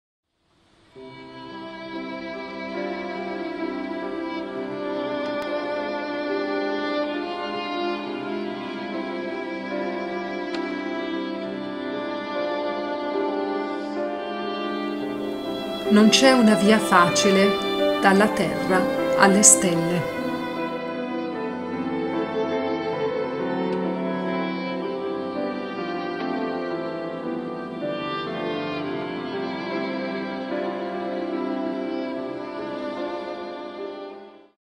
AFORISMI - Lucio Anneo Seneca (4 a.C. - 65 d.C.)
AFORISMI - Lucio Anneo Seneca (4 a.C. - 65 d.C.), filosofo e drammaturgo latino

"Non c'è una via facile
dalla terra alle stelle"